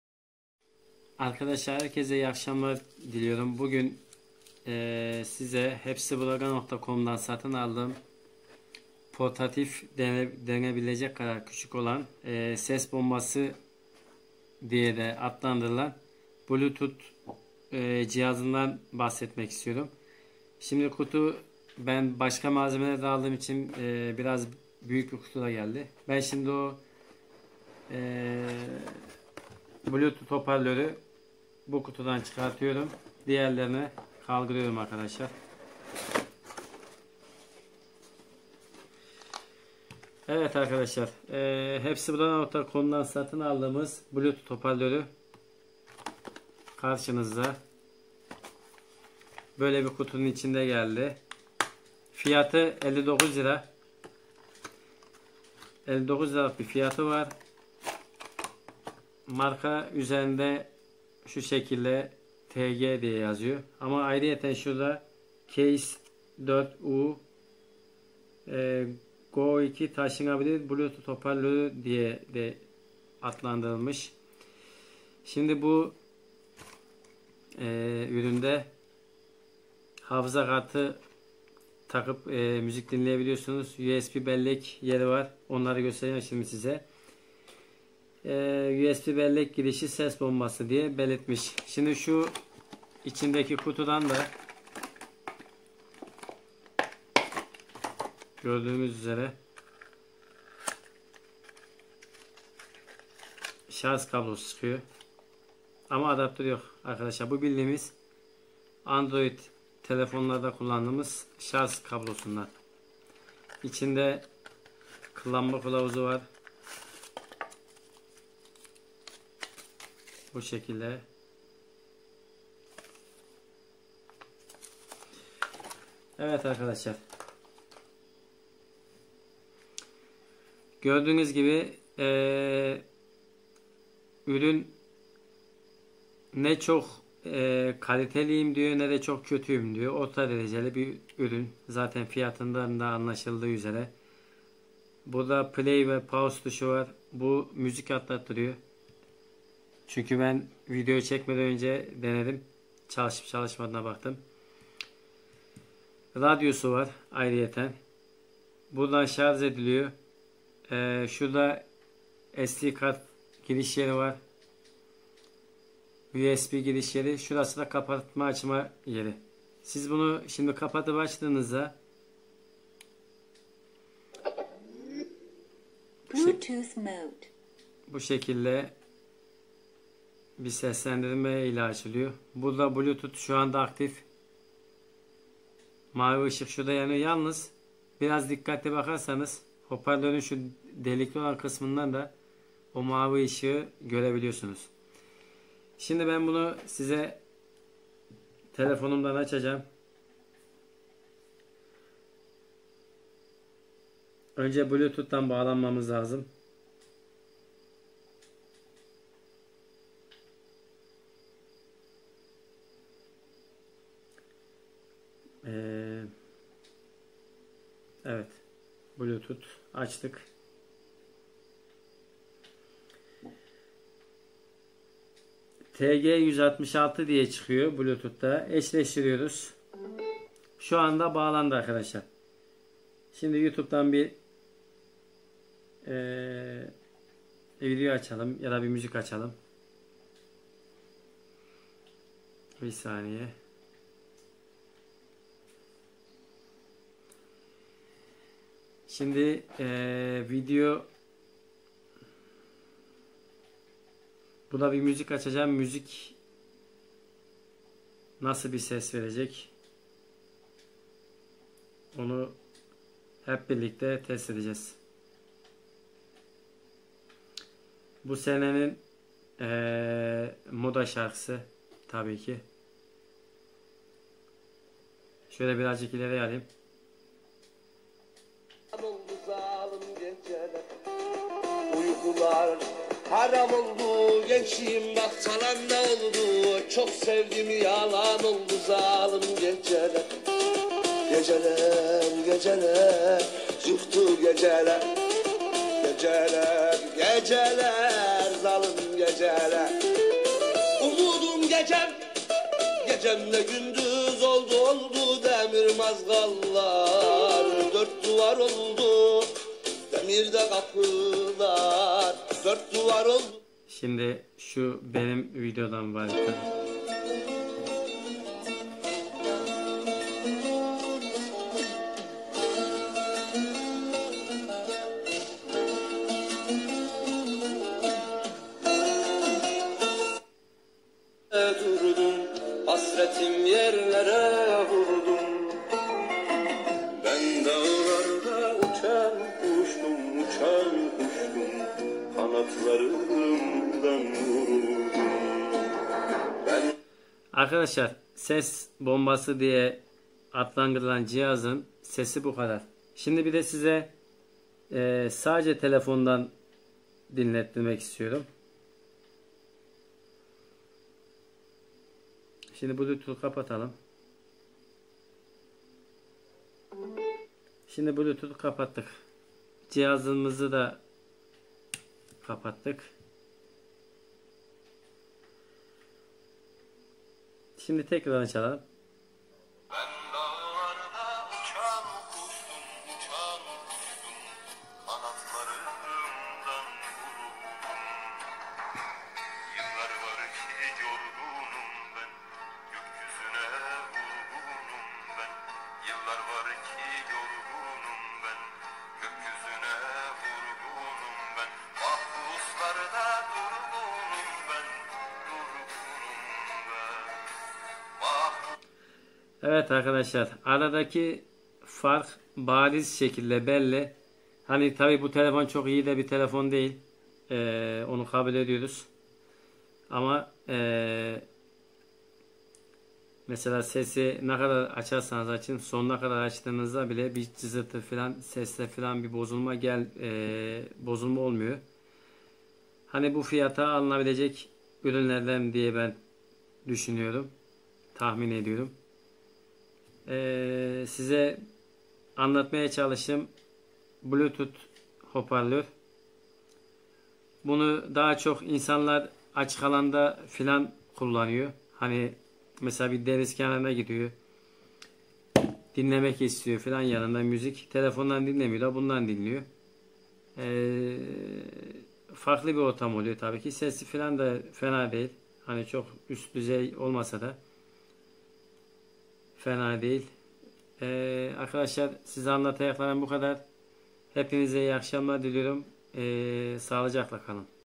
Ses bombasi !
hepsiburada.com dan satin aldigim bluetood mini hoparlör hakkinda mini bilgiler. Iyi seyirler